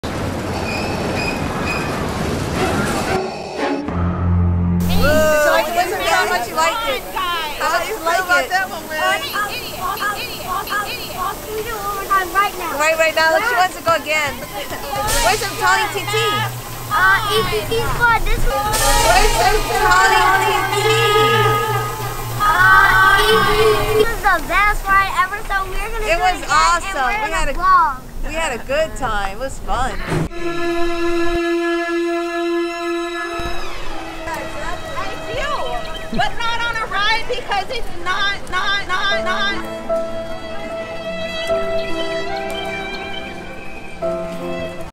Crazy Karen GONE WILD on a roller coaster
Crazy Karen Caught on Camera Yelling while on Thunder Mountain Railroad.